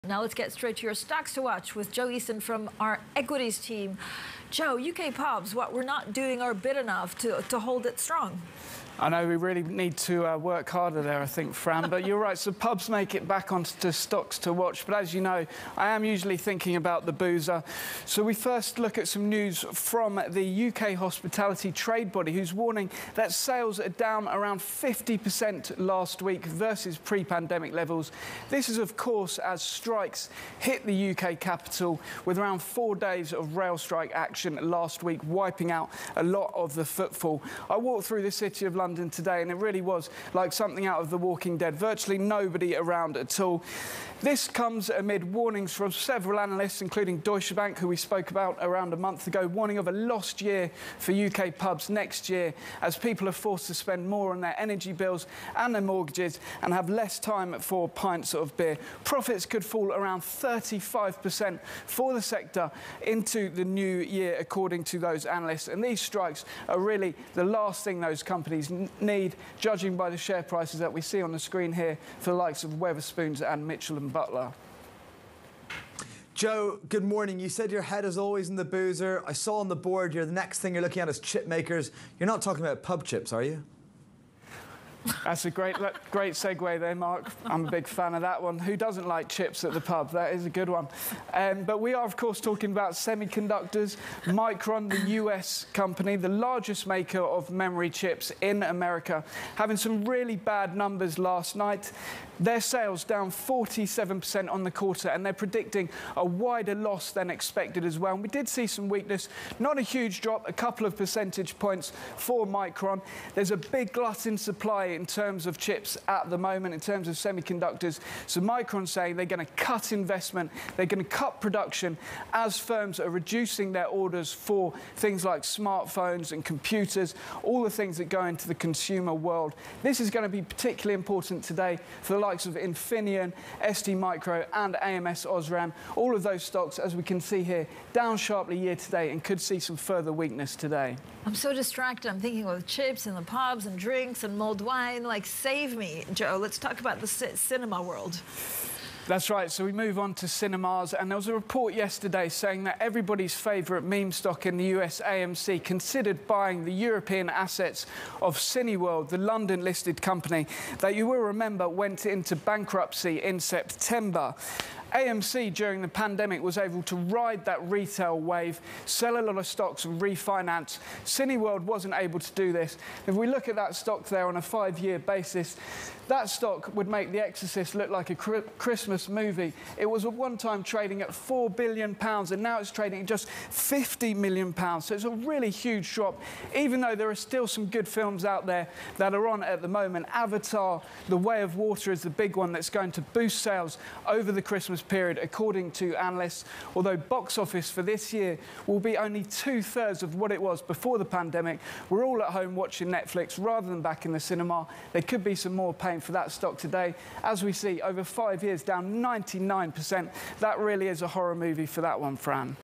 Stocks to Watch: Leisure Stocks, Micron, Cinemas
Bloomberg's Joe Easton has a rundown of the stocks to keep an eye on today, including Micron Technology Inc. and Cineworld Group Plc. He speaks on Bloomberg Television.